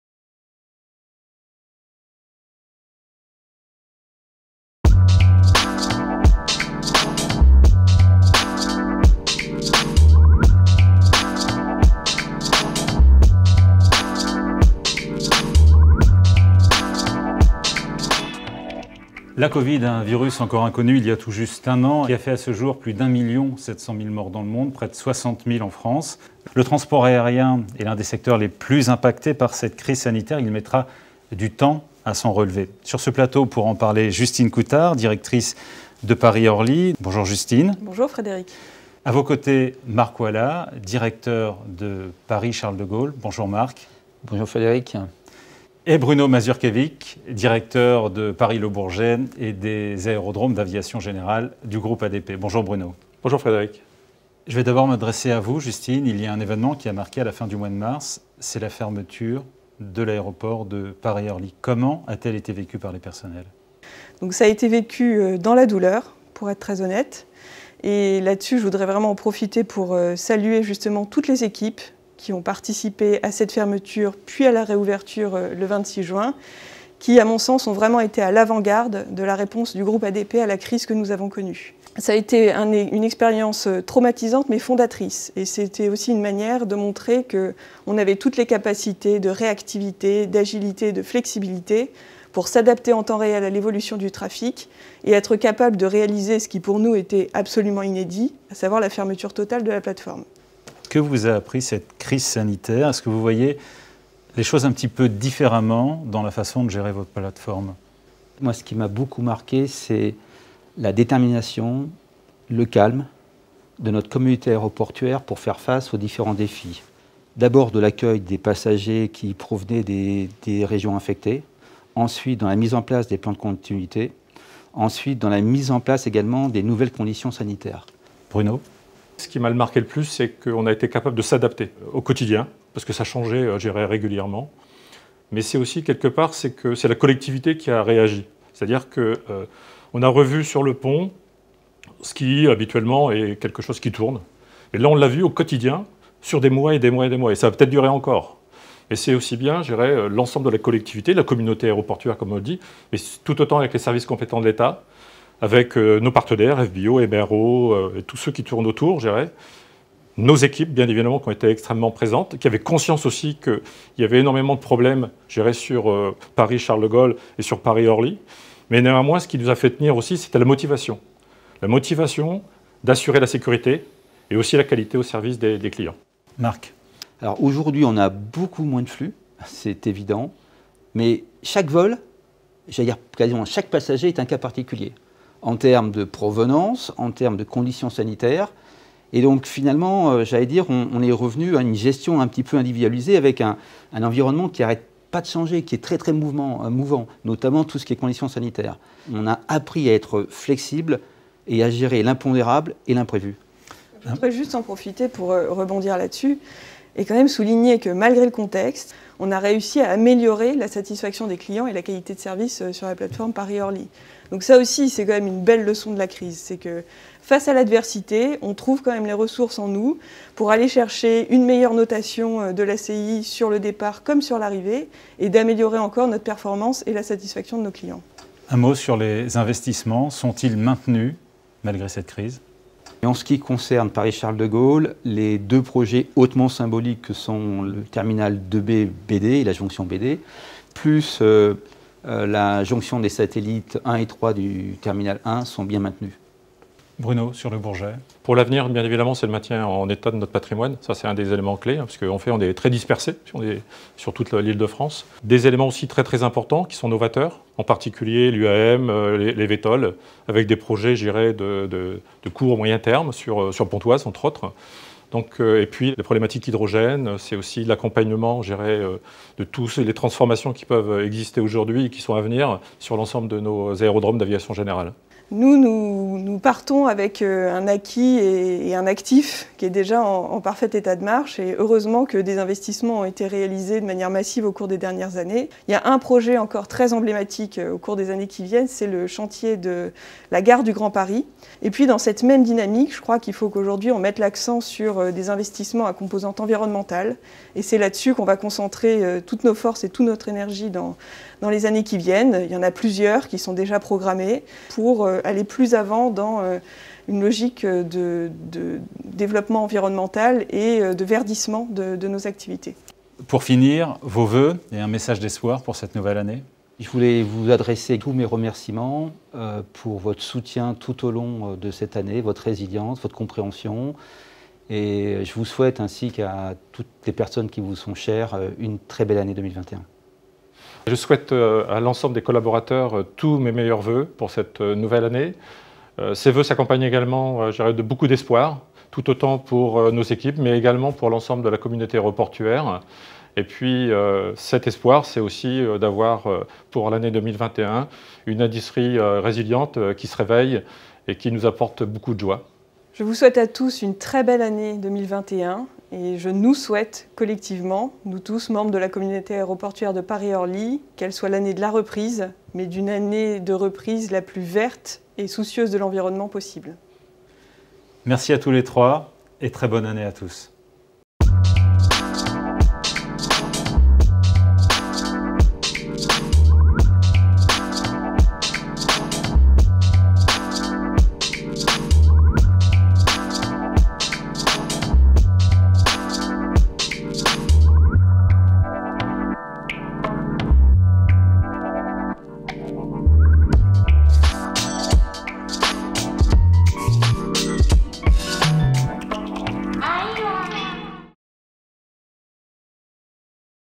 Season's greetings 2021 of Parisian airports directors
In 2021, let's continue together to share new horizons